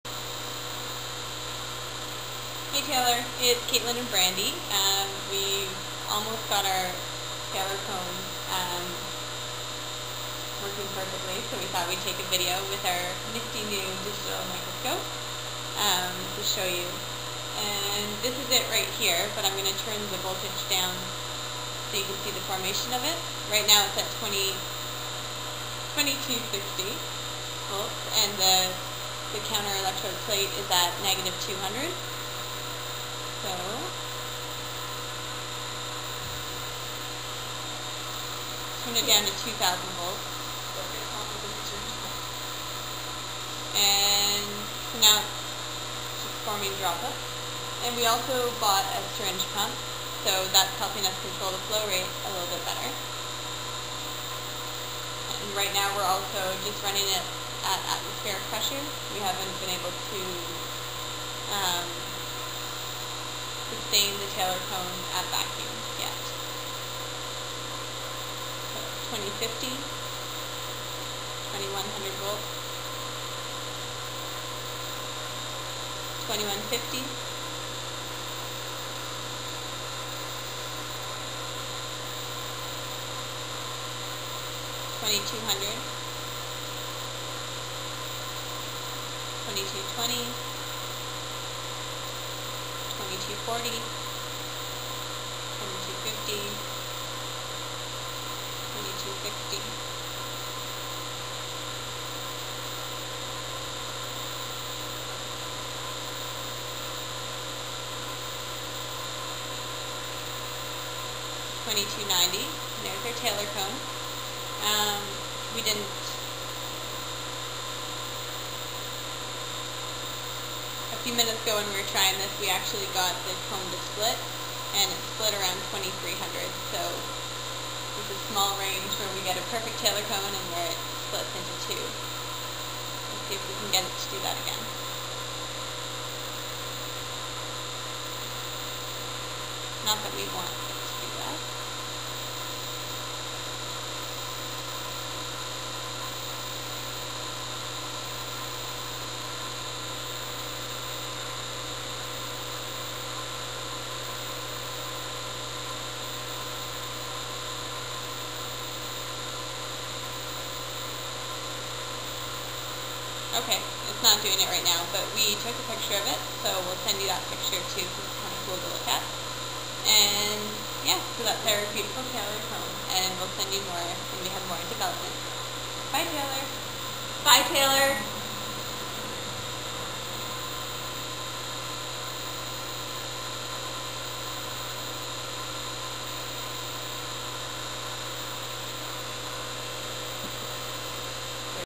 Electrospray Ionization with Syringe Pump
Working Electrospray with flow rate of 10uL/min using syringe pump